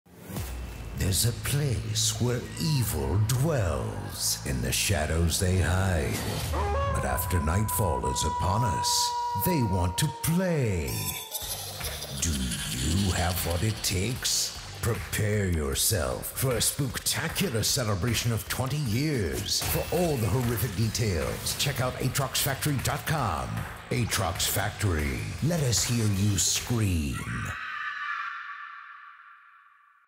Atrox Factory 2022
Atrox Factory in Leeds, Alabama is celebrating is 20th Season of Scares.